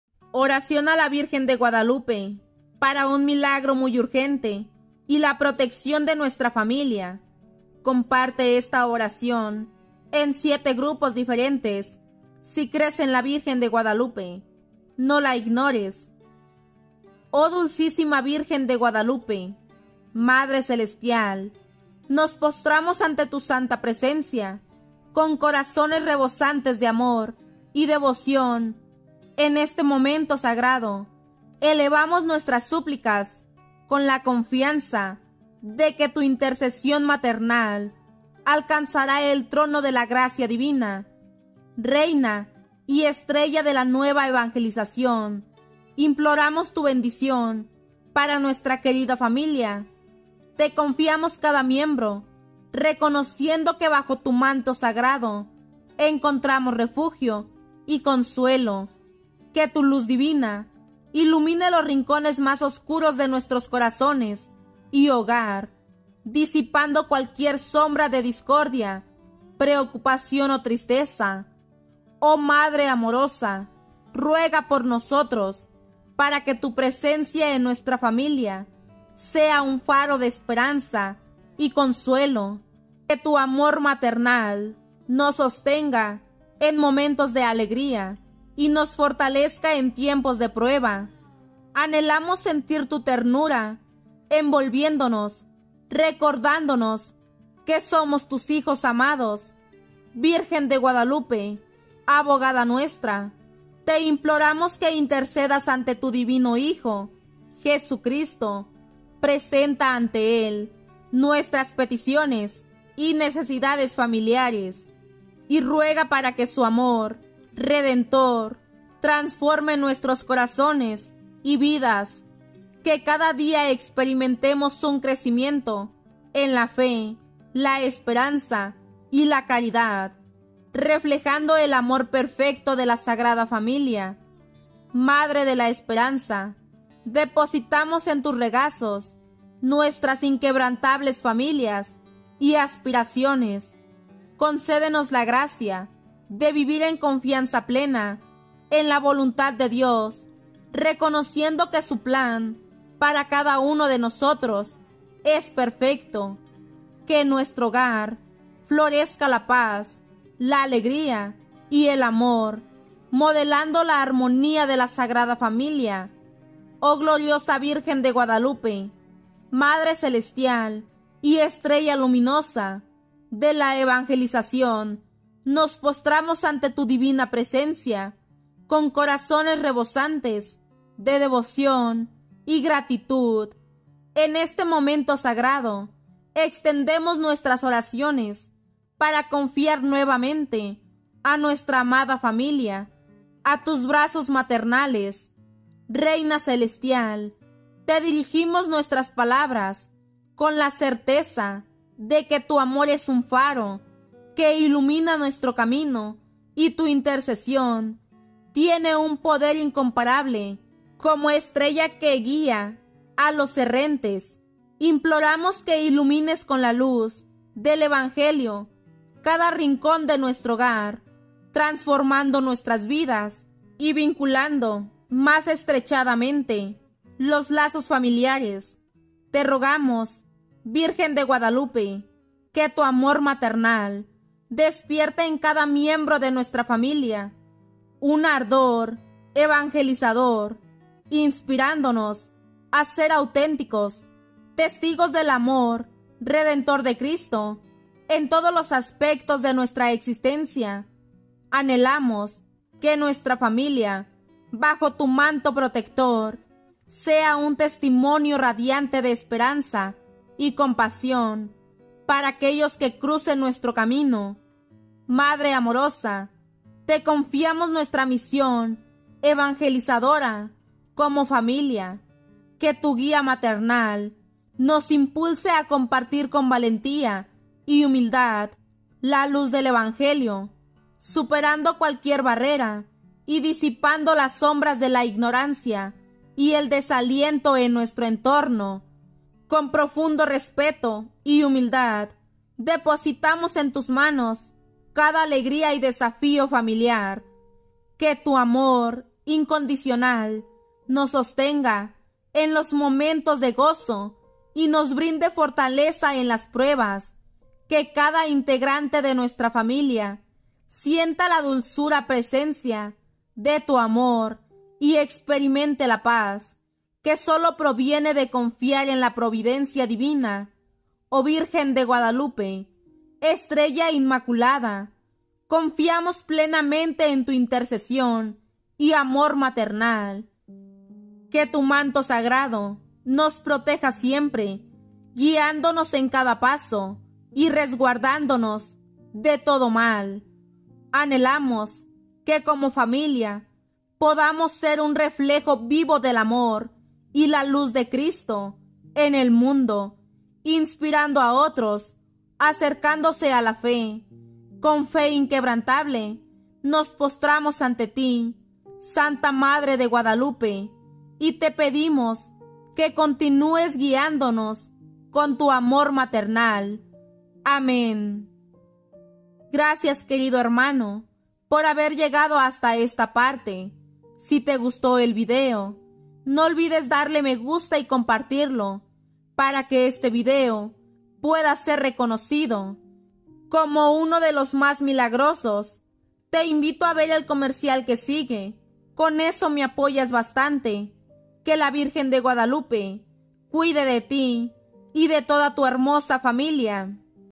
ORACION A LA VIRGEN DE GUADALUPE PARA UN MILAGRO MUY URGENTE Y LA PROTECCION DE NUESTRA FAMILIA
PIDE TU MILAGRO URGENTE  Y LA PROTECCION DE NUESTRA FAMILIA A LA SANTISIMA VIRGEN DE GUADALUPE CON ESTA MILAGROSA ORACION